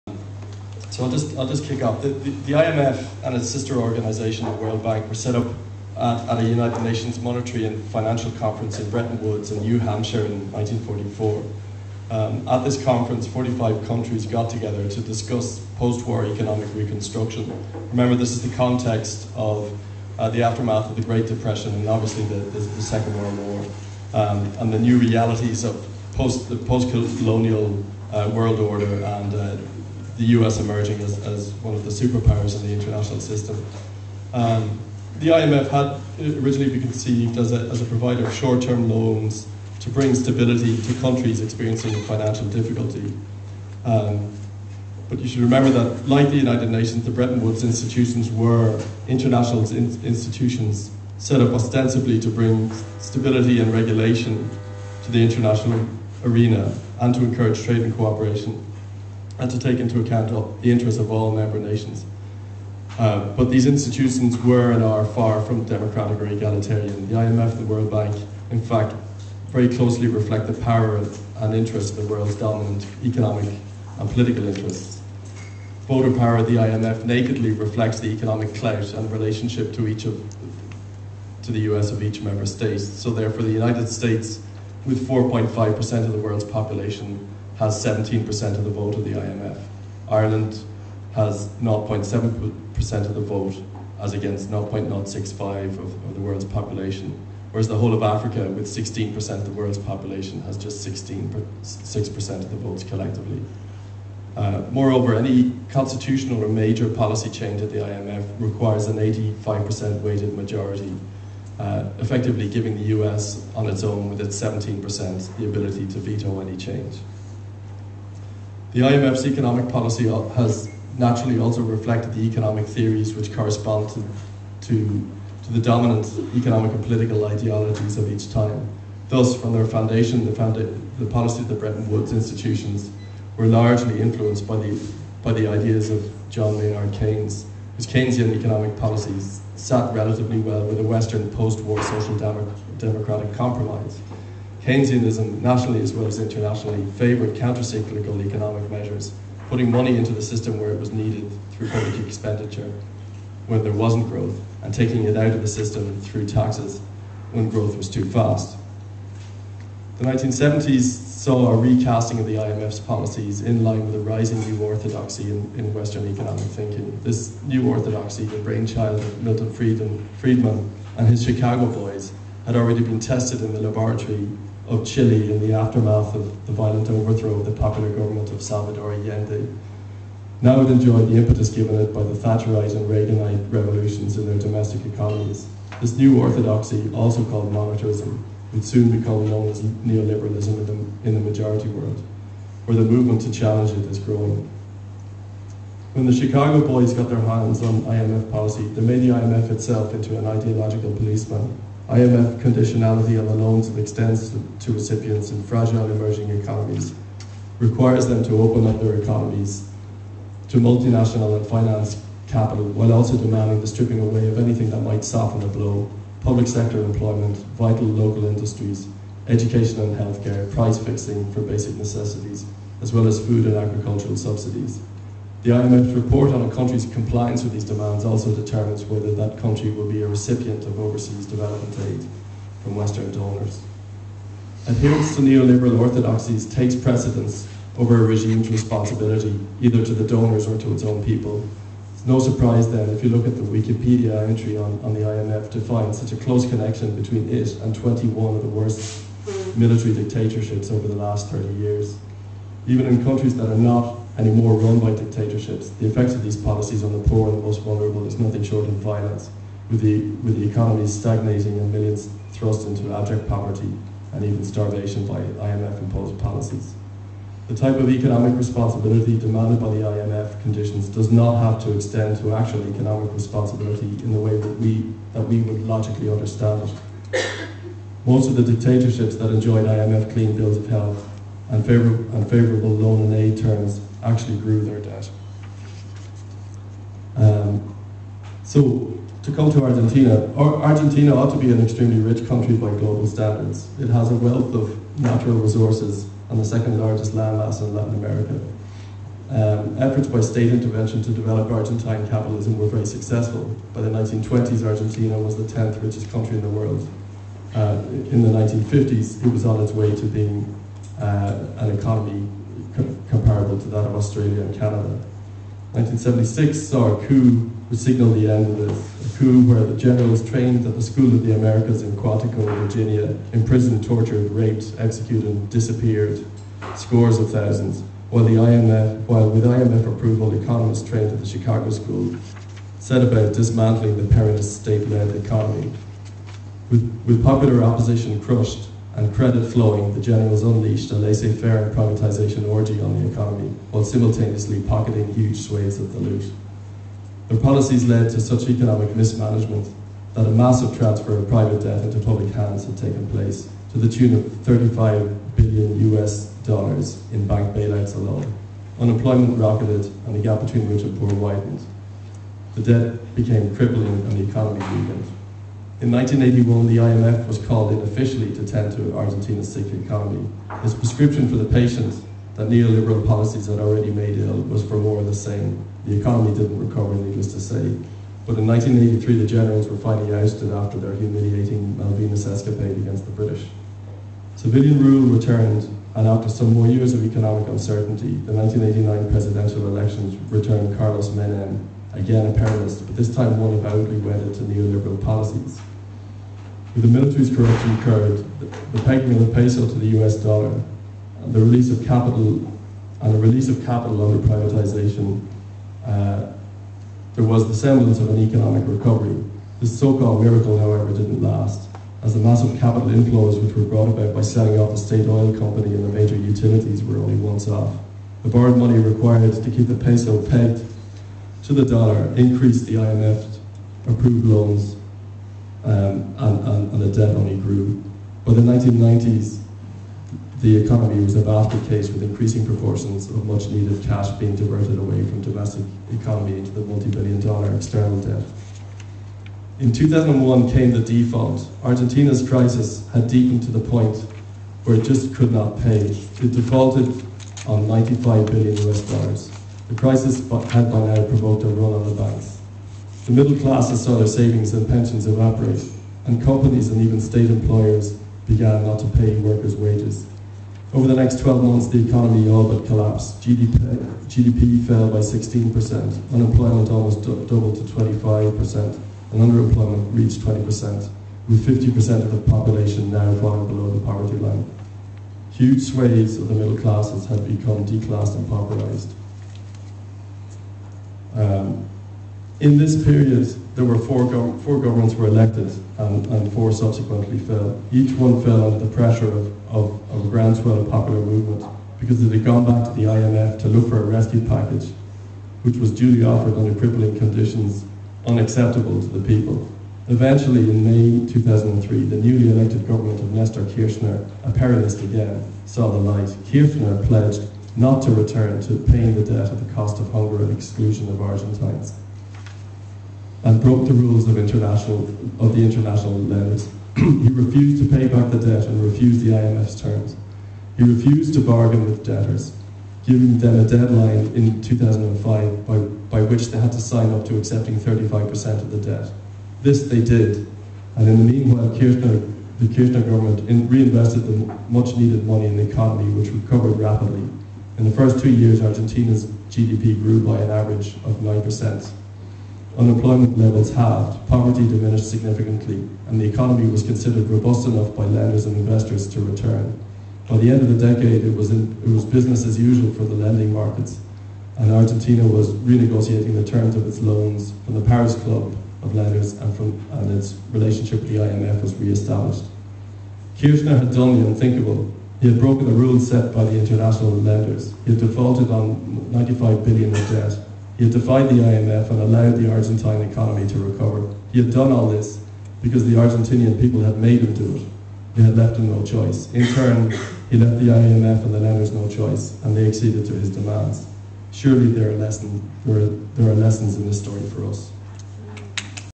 Shane O Curry, LASC
Shane o Curry talks in details about Argentina's default on its debt - a lesson for Ireland?